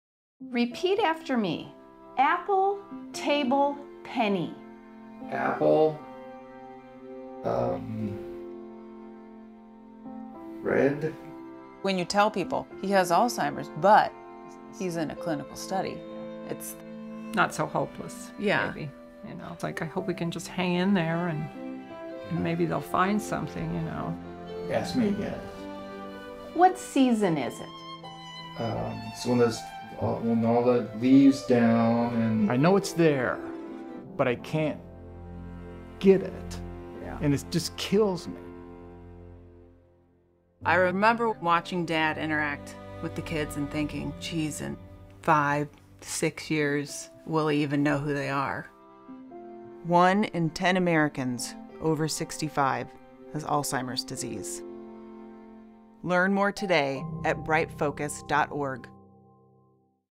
The Impact of Alzheimer's: Pasquale's Story (60 second PSA)
Watch Stories About the Impact of Alzheimer's Disease

The Impact of Alzheimer’s PSA series depict the powerful, first-person accounts of families impacted by Alzheimer’s disease.  Learn more about Pasquale, who enrolled in a clinical after trial receiving after his Alzheimer’s diagnosis.